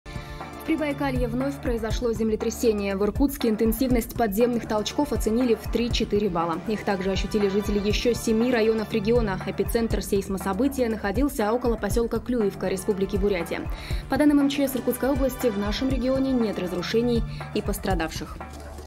Землетрясение ощутили жители 7 районов Иркутской области
В Прибайкалье вновь произошло землетрясение. В Иркутске интенсивность подземных толчков оценили в 3-4 балла. Их также ощутили жители ещё 7 районов региона. Эпицентр сейсмособытия находился около посёлка Клюевка Республики Бурятия. По данным МЧС Иркутской области, в нашем регионе нет разрушений и пострадавших.